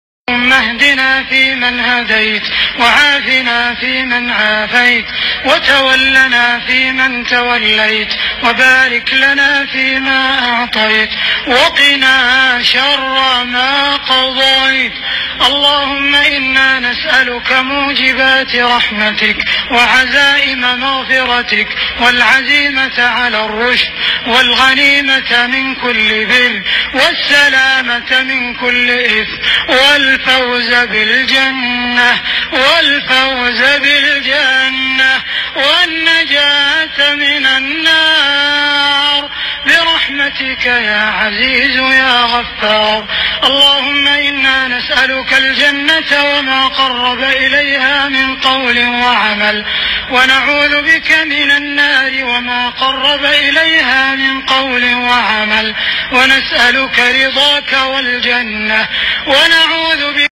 Breathtaking drone footage of Masjid Al Haram in Mecca is simply stunning!
This breathtaking drone footage of Masjid Al Haram in Mecca is simply stunning! Shows the thousands of visitors to the house of Allah, praying and circumambulating around the Kabah.